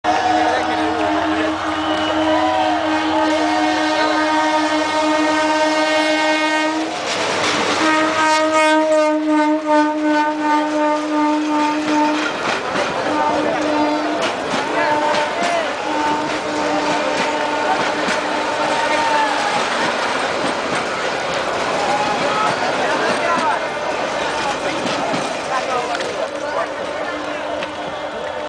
Haunted Crowded Train in the World- Bangladesh Railway
Bishwa Ijtema Special Train 2013/ Bishwa Ijtema Akhiri Munajat Special Train Services 2013